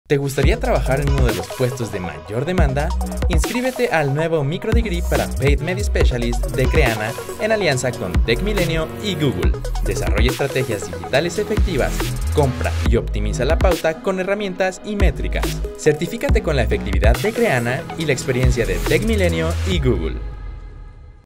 Nuevo: MicroDegree para Paid Media Specialist | Crehana - CDC Tecmilenio - Google
¡Acabamos de lanzar el nuevo MicroDegree para Paid Media Specialist!

Si siempre quisiste trabajar en áreas de Performance Marketing, Paid Media o quieres aprender a crear estrategias efectivas en medios digitales pagados para maximizar el retorno de tu inversión, este programa online es para ti. Lo co-creamos junto al CDC Tecmilenio, con el apoyo de Google, para que logres encontrar ese ascenso o puesto que buscas. Con este Programa, vivirás una experiencia de 4 meses bajo una metodología de desarrollo de proyectos y mentorías en vivo.

¡Aprovecha e inscríbete ahora!